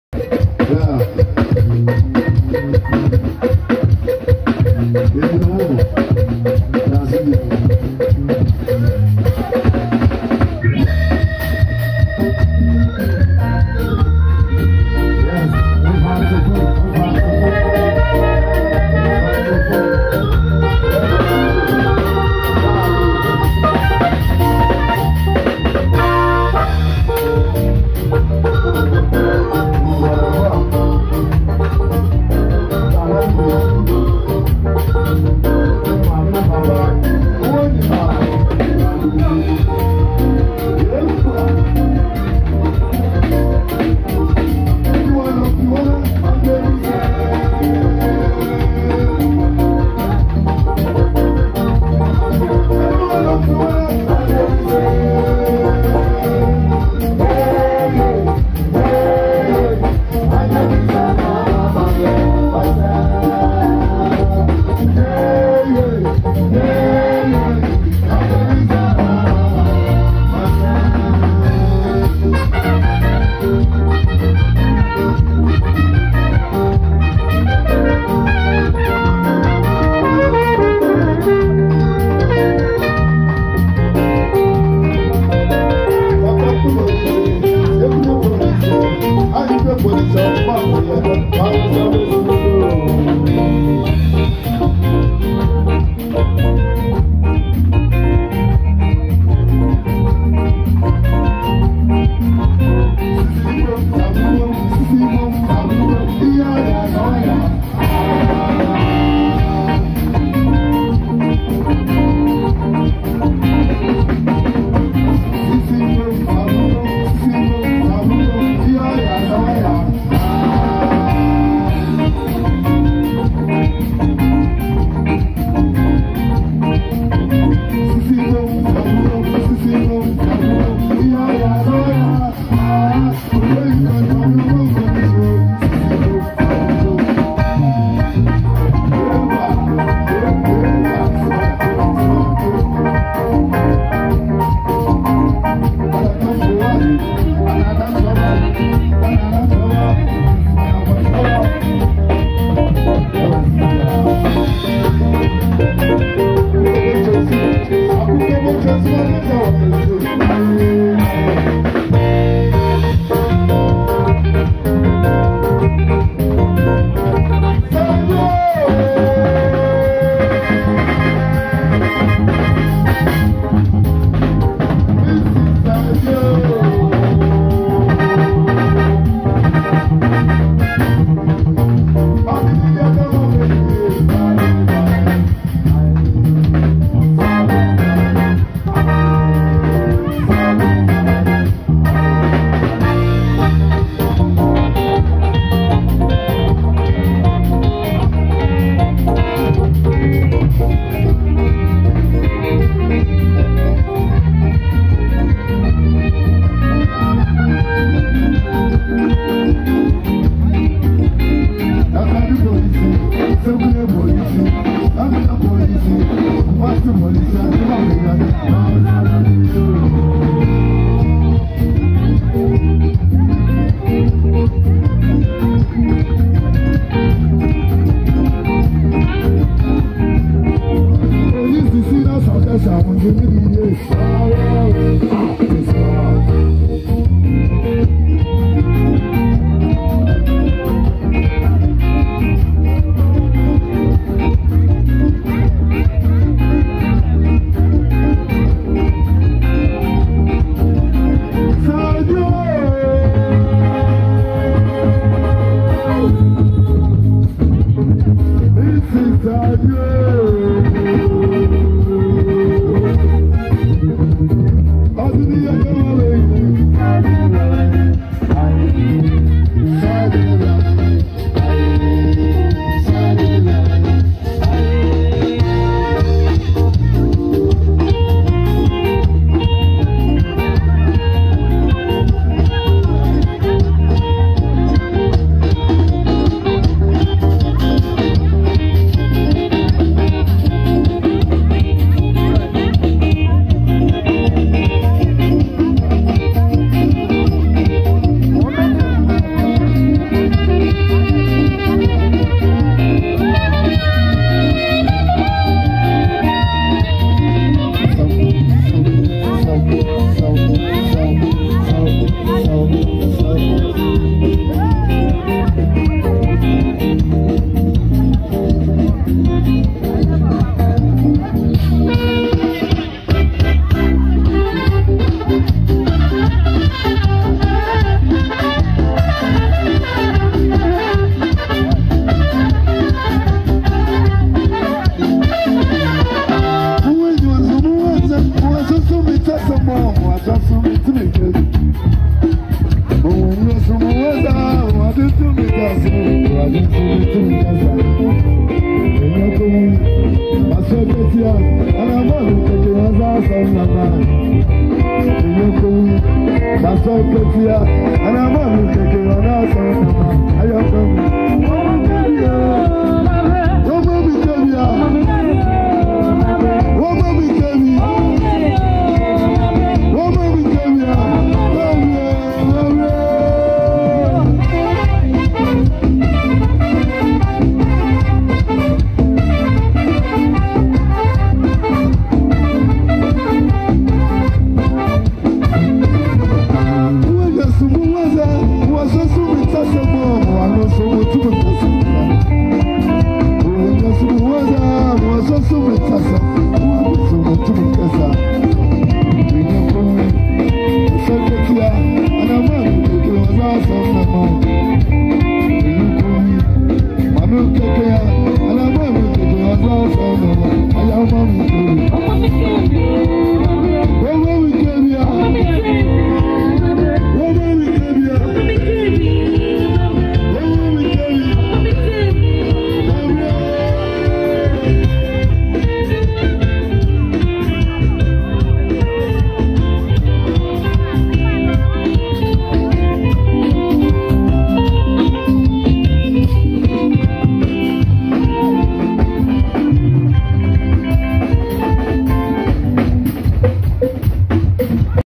Paapa Yankson's hit songs performed by Prisons band nicely
Prisons band has always been fantastic in their performances whenever they mount the stage